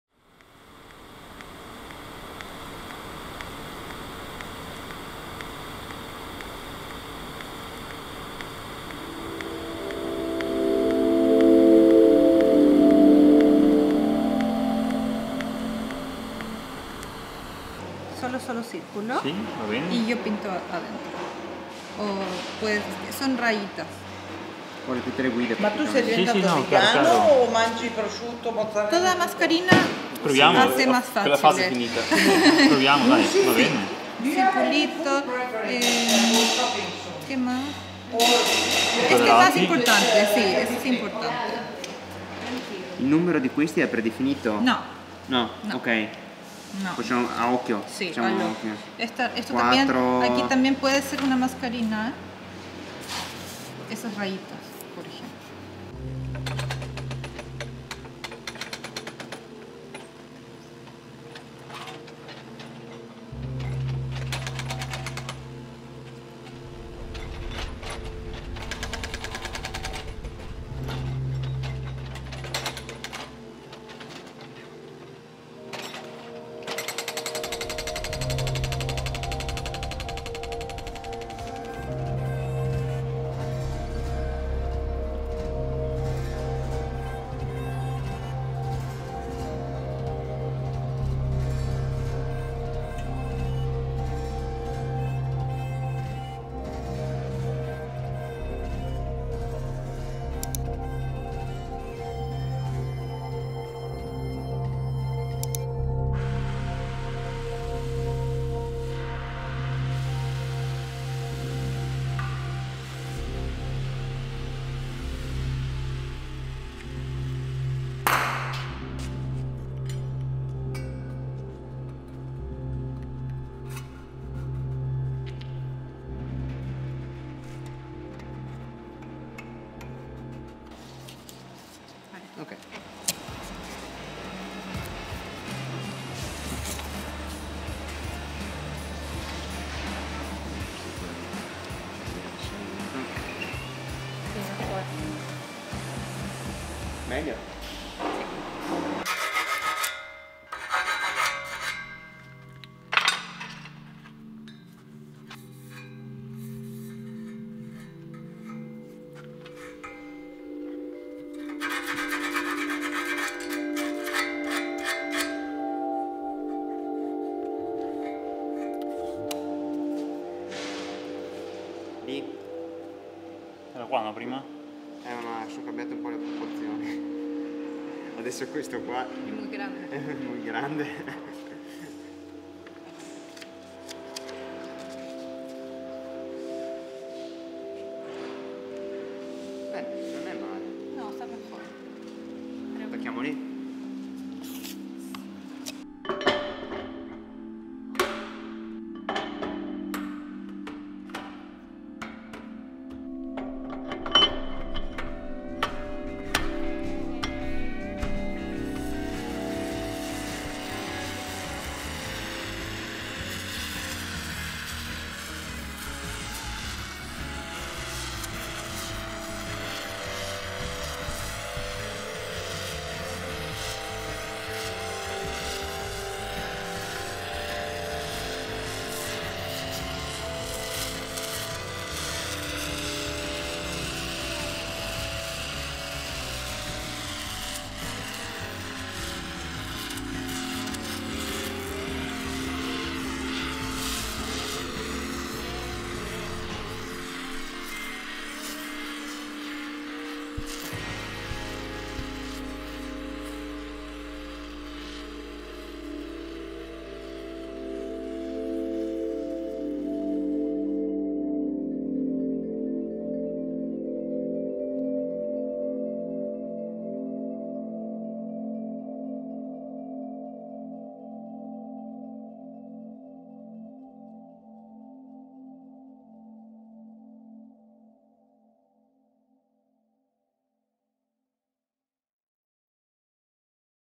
Beyond the Frame With Artist Muriel Gallardo Weinstein
In 'Beyond the Frame',  multi-disciplinary artist Muriel Gallardo Weinstein visits the Bottega to co-create a Ciavete.